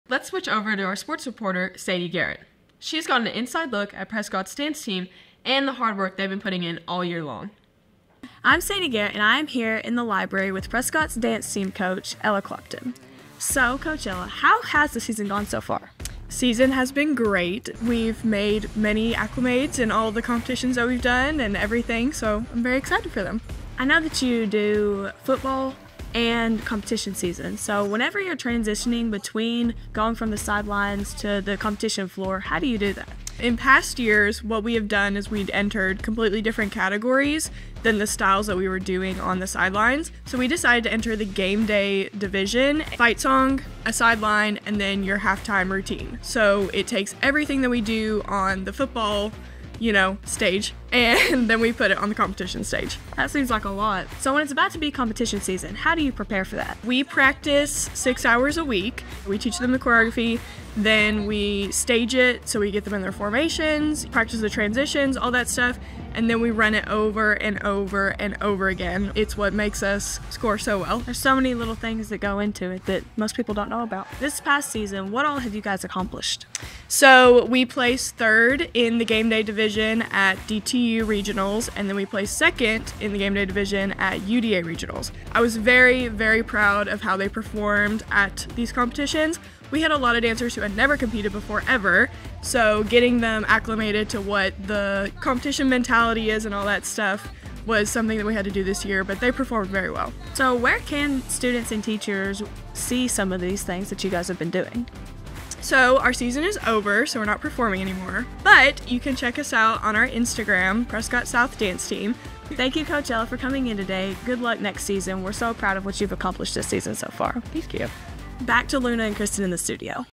Sports Report: Dance Team
Sports Reporter Sadie Garrett meets with Coach Ella Clapton to learn about all the work our Dance Team has been putting in all year.

Off-Camera Production Credits:
Lead Writer: Ava Wilson
Lead Editor: Dulcie Dickerson
Camera Operator: Katelyn Crabtree
Assistant Director: Leeton Norris